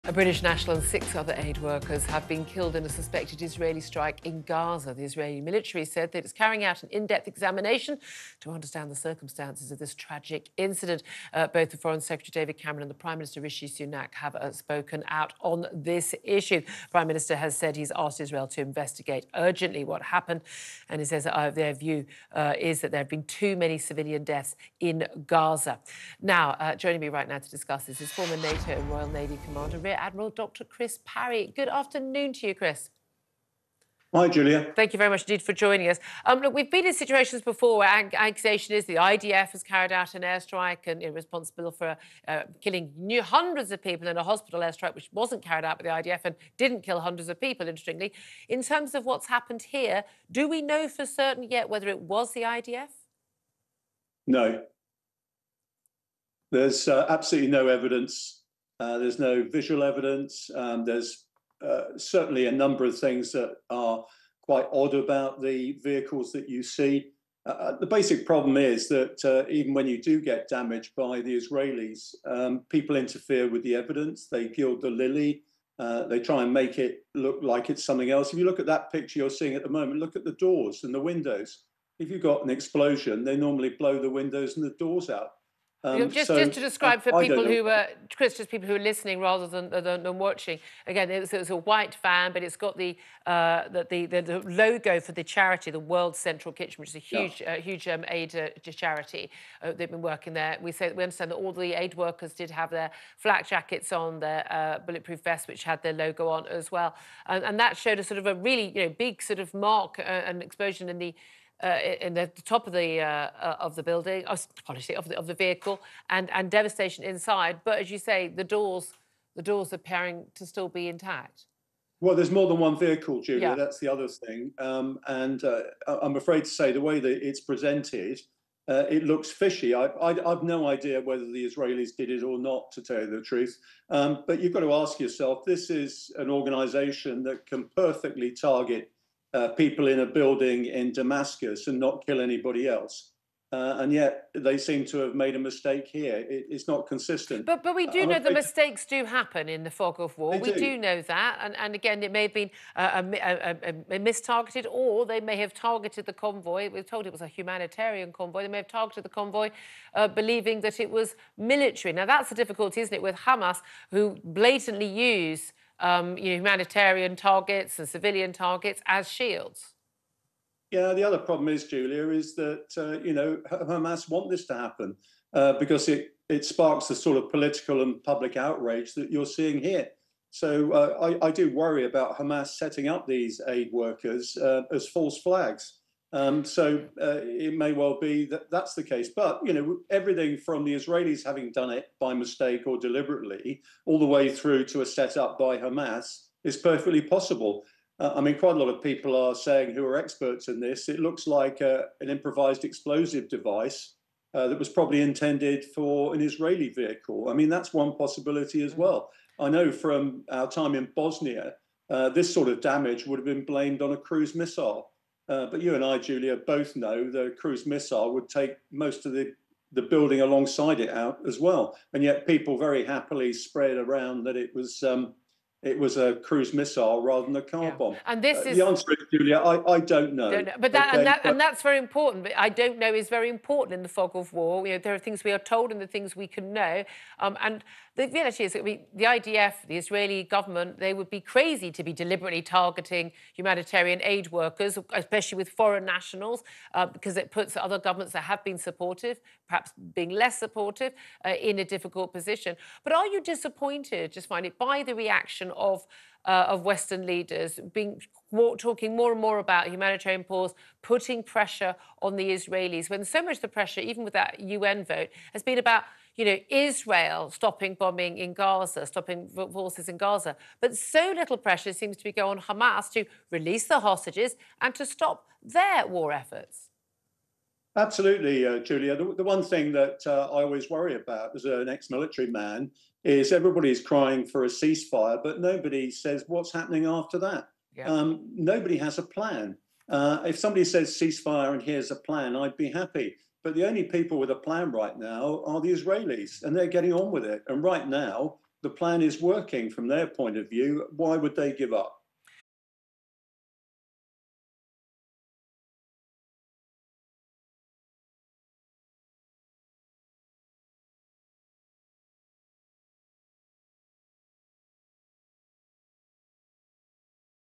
“No Evidence To Suggest IDF Were Responsible” | British Aid Workers Killed In Gaza
Three British citizens were among seven killed in a suspected Israeli airstrike in Gaza while working for World Central Kitchen.

Prime Minister Rishi Sunak and Foreign Secretary David Cameron called for an investigation.

Despite coordinating with the Israeli military, World Central Kitchen's convoy was hit, including two clearly marked aid vehicles.

Damage to a vehicle labeled "World Central Kitchen" was captured in images and footage from the scene.

Julia Hartley-Brewer speaks with ex-NATO commander, Dr Chris Parry CBE who discusses the validity of the IDF’s involvement in the killings.